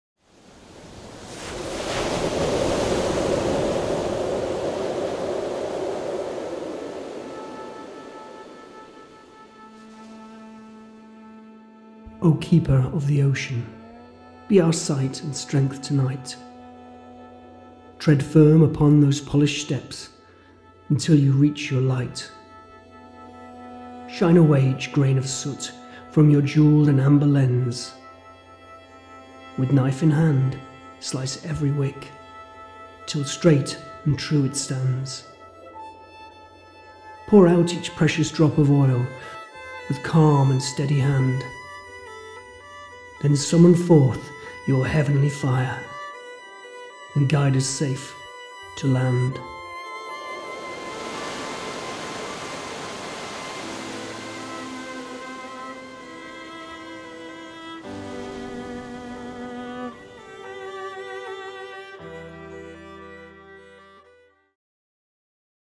4. Keeper Of The Ocean
A beautiful photo of a light house from Roy Barry with a few words added by Dave Cheetham, to make it really shine.